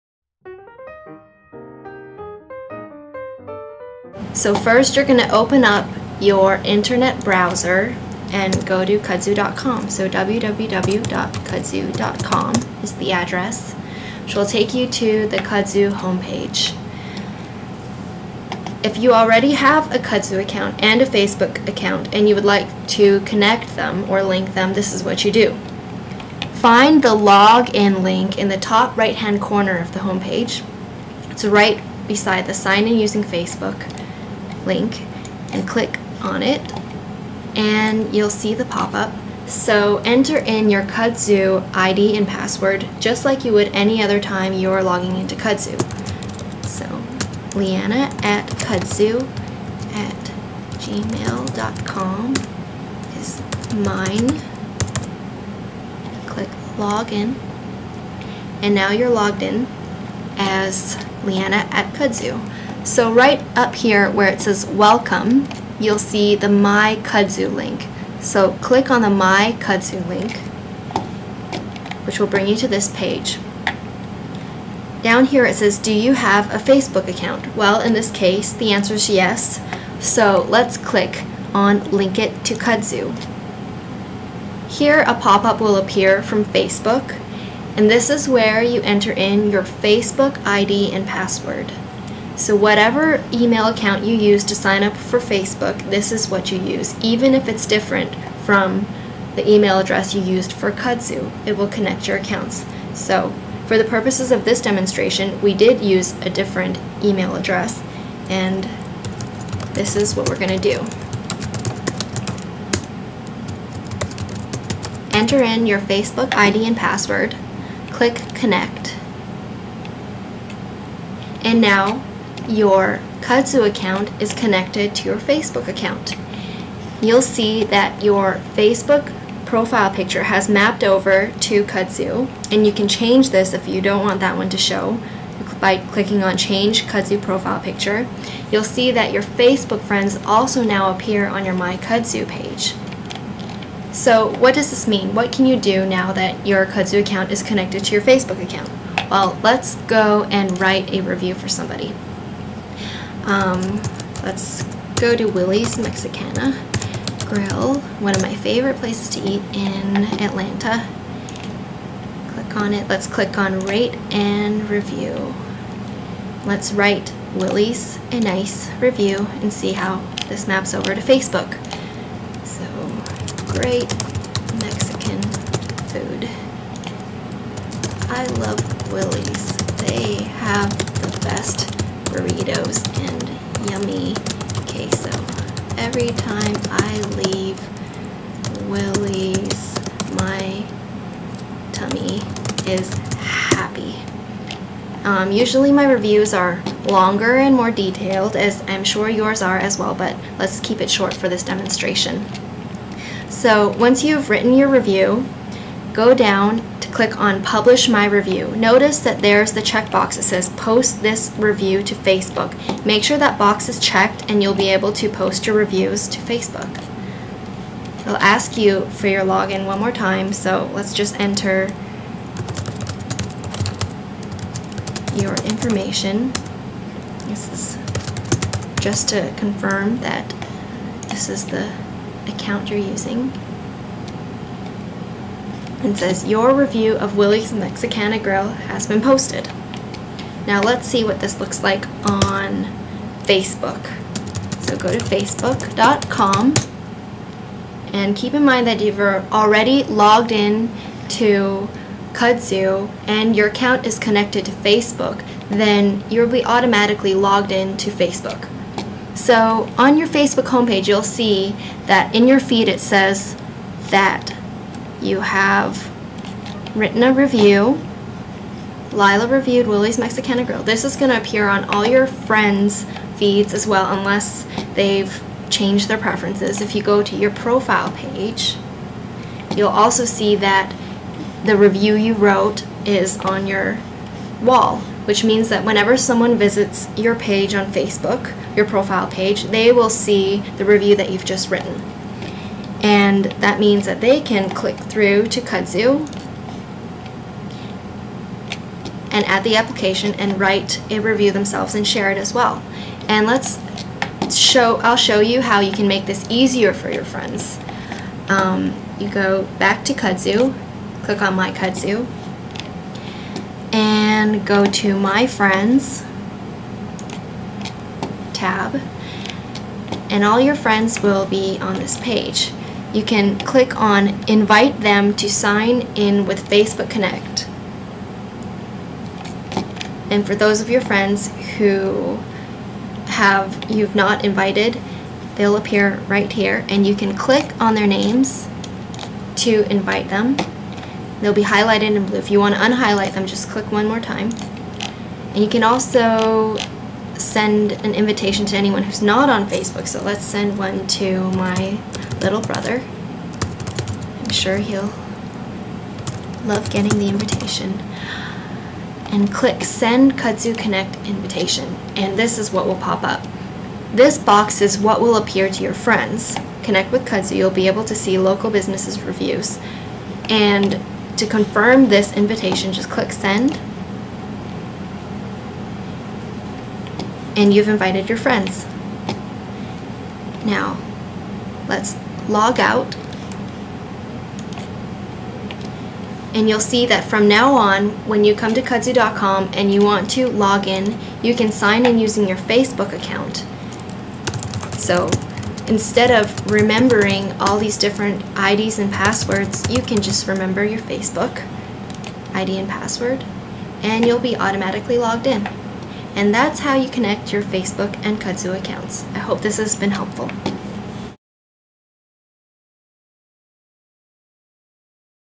Facebook Connect Tutorial: Connecting your Kudzu.com and Facebook Accounts
How to use Facebook Connect on Kudzu, and connect your Facebook and Kudzu accounts.